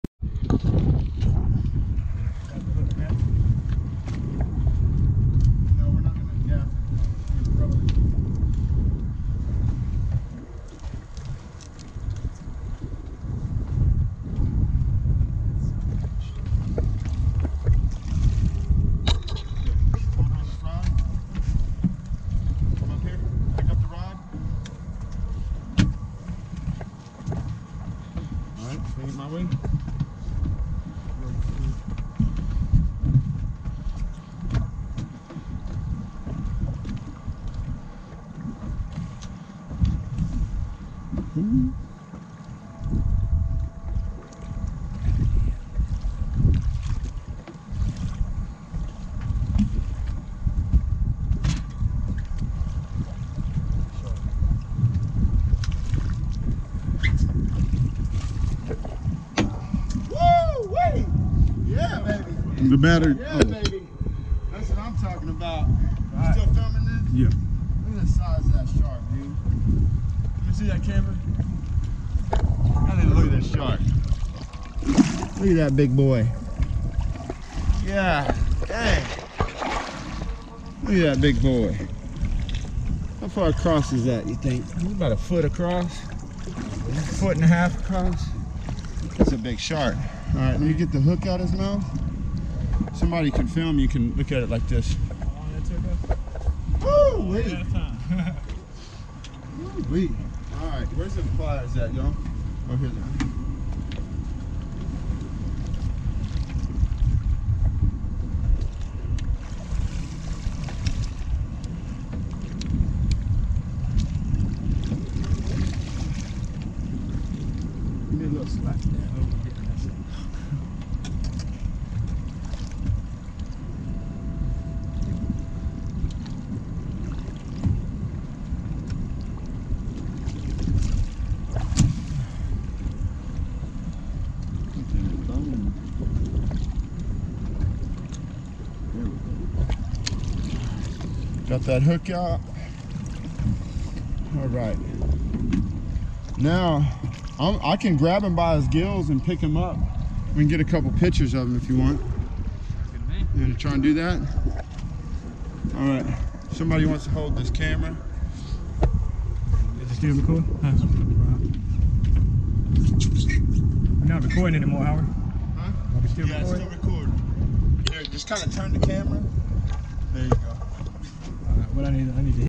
HUGE BULL SHARK FISHING IN GALVESTON
This video was from a few years ago when I was working for Get Hooked Charters in Galveston (gotta give credit, even though this is my video). The customer couldn't get the shark to come up and he asked me to take over. This is a HUGE BULL SHARK and so powerful. It's a really amazing feeling to lift up a shark of this size on a fairly light rod and reel!